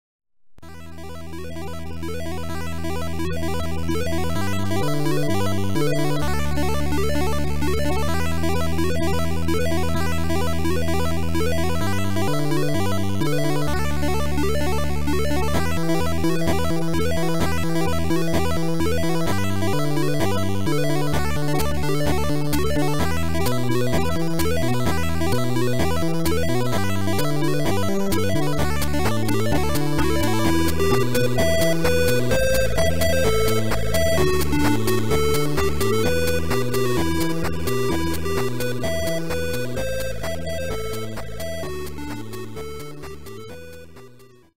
WARNING video contains flashing images - Medicall Ambulance - CPC Course on Communications
Medicall Ambulance CPC Course on Communications, review radio communications, ASHICE and METHANE Messaging along with tips for PCR completion and advice on calling 999 or 112 for an emergency ambulance